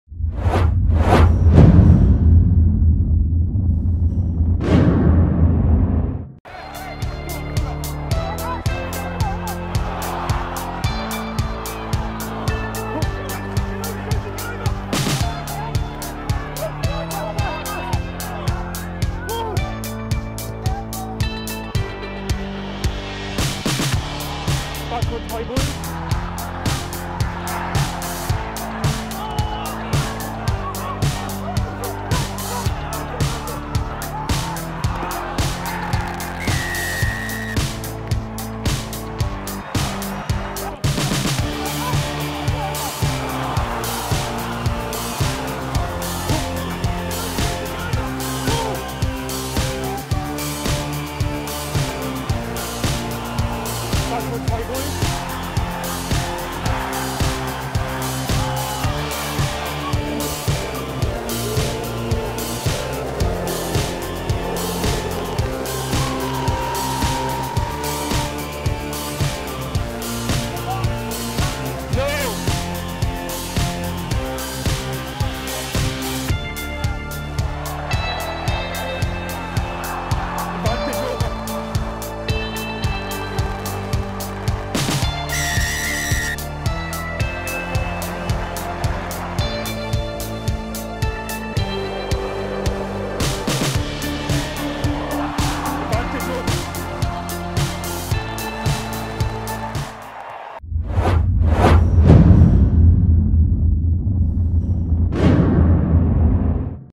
Wasps go the length and Varndell gets a gift v Grenoble
London Wasps scored two tries as they beat Grenoble 32-12 at Adams Park in the Amlin Challenge Cup.

The first try was a great team effort that saw James Haskell bundle his way over the line while the second was a bit of a gift for Tom Varndell, who returned to the side after 3 months on the sidelines.